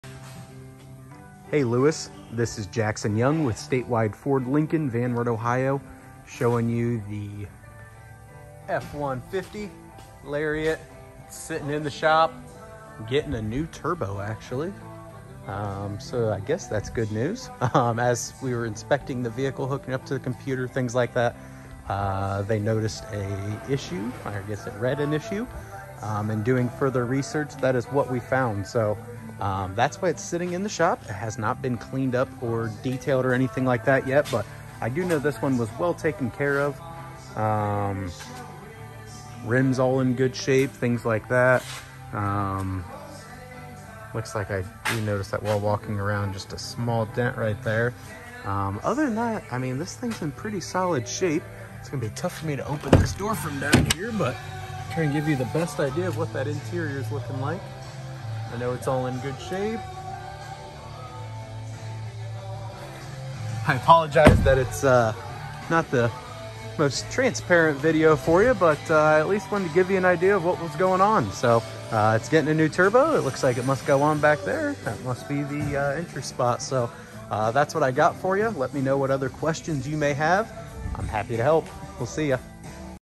2016 Ford F-150 Lariat for Luis
Jackson showing the 2016 F-150 Lariat at Statewide Ford.